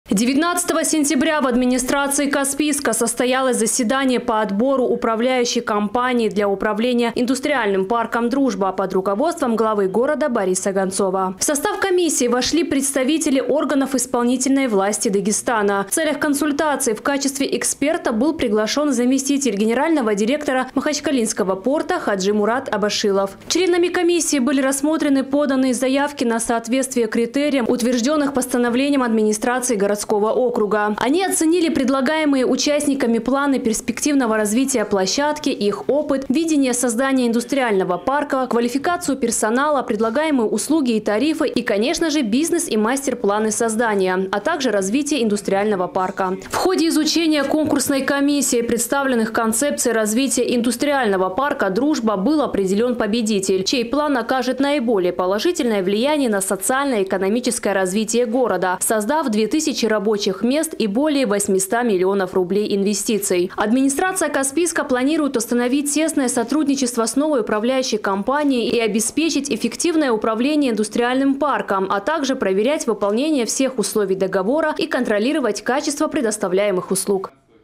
В Каспийске выбрали УК для управления индустриальным парком «Дружба»
19 сентября в администрации Каспийска состоялось заседание по отбору управляющей компании для управления индустриальным парком «Дружба» под руководством главы города Бориса Гонцова. В состав комиссии вошли представители органов исполнительной власти Дагестана. В целях консультации в качестве эксперта был приглашен заместитель гендиректора махачкалинского порта Хаджи-Мурад Абашилов.

Членами Комиссии были рассмотрены поданные заявки на соответствие критериям, утверждённых постановлением администрации городского округа.